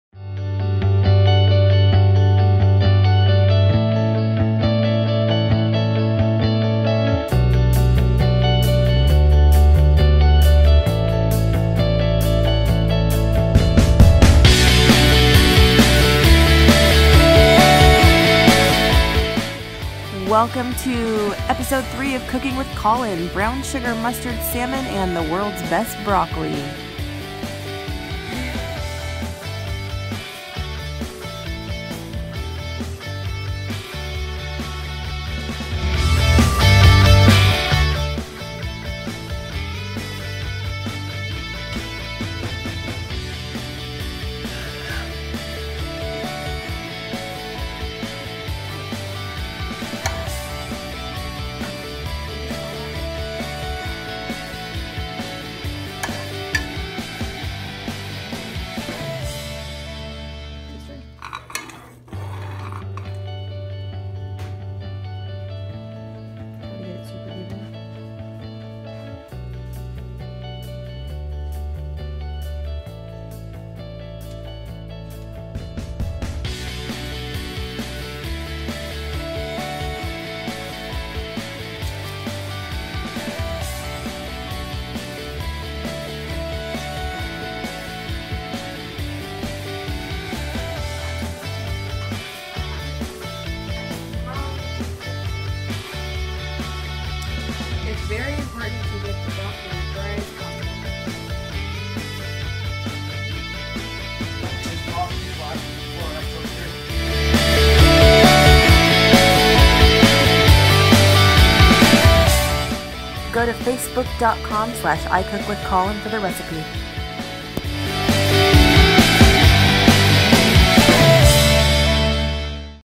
Cooking With Collin - Brown Sugar Dijon Salmon
Brown sugar Dijon salmon with the world's best broccoli! SAVE THIS VIDEO so you'll have the recipes! And please share. This was SO good I ate more broccoli than I ever have in my life!

For the salmon:
Sprinkle salmon fillet with garlic salt and bake at 425 for 10 minutes. While it's baking, add the following to a pot and bring to a boil:
2TBS butter
1TBS olive oil
3TBS brown sugar
1TBS honey
2TBS Dijon mustard (I like more)

Once it's boiling, your salmon should be ready to take out of the oven. Glaze the salmon with this mixture then broil it for 6 minutes on high.

For the broccoli:
Rinse and slice a bunch of trees, then get them as dry as possible. That's very important. Coat them with olive oil, add salt and pepper (I also added some garlic salt). Spread out on a cookie sheet and bake at 425 for 10 minutes. Flip those bad boys over and bake another 10 minutes. They will look burnt. Trust me, they're not. Those black parts are heavenly pockets of caramelized Yumminess that will make you eat way too much.

If you try these recipes out, please comment and let us know how you like them.

Cooking with Collin is a cooking show for kids and parents where my 12 year old son is learning how to cook and will share his tips and tricks with you along the way.